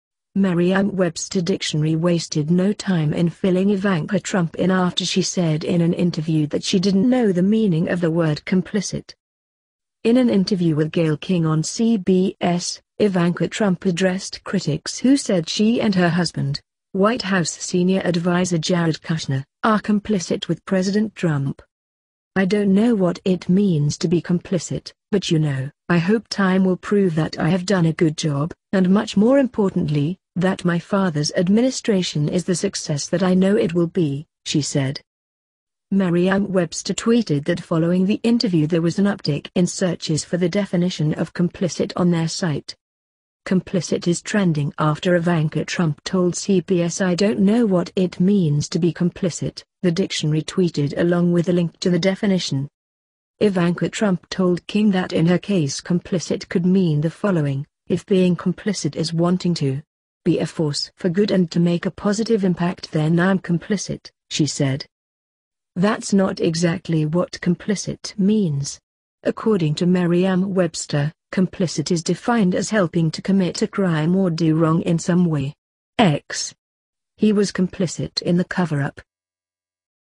Merriam-Webster defines complicit for Ivanka Trump
Merriam-Webster dictionary wasted no time in filling Ivanka Trump in after she said in an interview that she didn't know the meaning of the word complicit.

In an interview with Gayle King on CBS, Ivanka Trump addressed critics who said she and her husband, White House senior adviser Jared Kushner, are “complicit” with President Trump.

“I don’t know what it means to be complicit, but you know, I hope time will prove that I have done a good job, and much more importantly, that my father’s administration is the success that I know it will be,” she said.

Merriam-Webster tweeted that following the interview there was an uptick in searches for the definition of complicit on their site.

"'Complicit' is trending after Ivanka Trump told CBS "I don’t know what it means to be complicit,'" the dictionary tweeted along with a link to the definition.

Ivanka Trump told King that in her case complicit could mean the following: “If being complicit is wanting to... be a force for good and to make a positive impact then I’m complicit," she said.